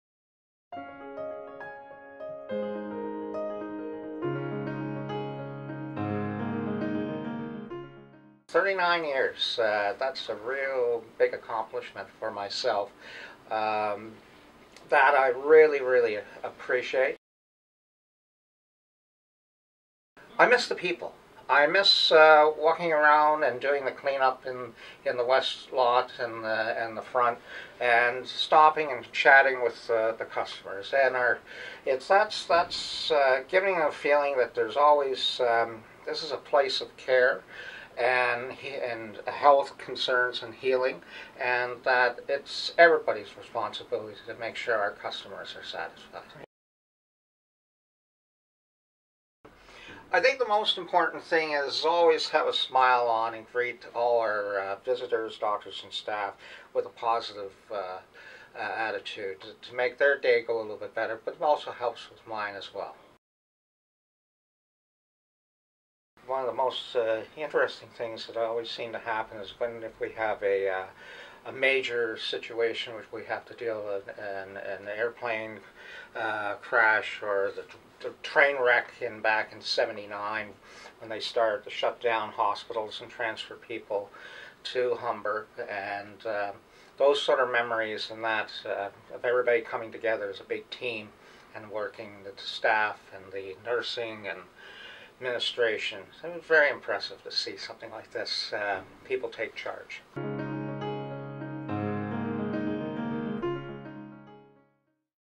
Ron Salisbury Retires After 39 Years
Ron Salisbury retires after 39 years at Humber River Hospital. Here he shares his thoughts about leaving.